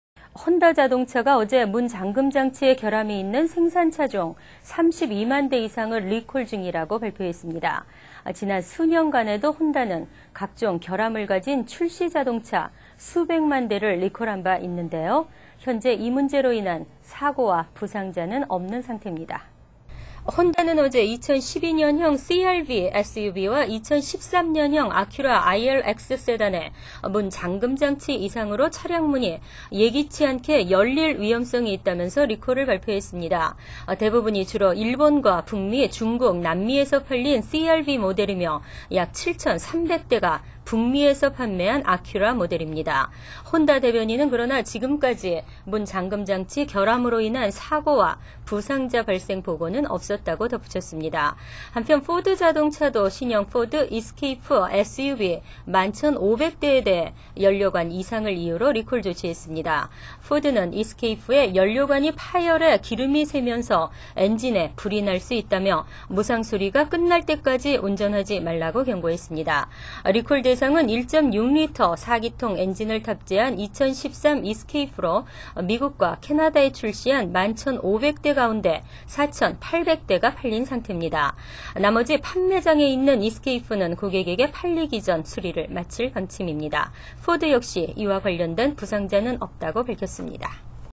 혼다, 포드 32만여대 리콜
혼다자동차가 어제 문 잠금장치에 결함이 있는 생산 차종 32만대 이상을 리콜 중이라고 발표했습니다.

지난 수년간에도 혼다는 각종 결함을 가진 출시 자동차 수백만 대를 리콜한 바 있는데요.

현재 이 문제로 인한 사고와 부상자는 없는 상태입니다.

혼다는 어제 2012년 형 CR-V SUV와 2013년 형 아큐라 ILX 세단에 문 잠금장치 이상으로 차량 문이 예기치 않게 열릴 위험성이 있다면서 리콜을 발표했습니다.

대부분이 주로 일본과 북미, 중국, 남미에서 팔린 CR-V 모델이며 약 7천 300대가 북미에서 판매한 아큐라 모델입니다.

혼다 대변인은 그러나 지금까지 문 잠금장치 결함으로 인한 사고와 부상자 발생 보고는 없었다고 덧붙였습니다.

한편 포드 자동차도 신형 포드 이스케이프 SUV 1만 1천 500대에 대해 연료관 이상을 이유로 리콜 조치했습니다.

포드는 이스케이프의 연료관이 파열해 기름이 새면서 엔진에 불이 날 수 있다며 무상수리가 끝날 때까지 운전하지 말라고 경고했습니다.

리콜 대상은 1.6리터 4기통 엔진을 탑재한 2013 이스케이프로 미국과 캐나다에 출시한 1만 1천 500대 가운데 4천 800대가 팔린 상태입니다.

나머지 판매장에 있는 이스케이프는 고객에게 팔리기 전 수리를 마칠 방침입니다.

포드 역시 이와 관련된 부상자는 없다고 밝혔습니다.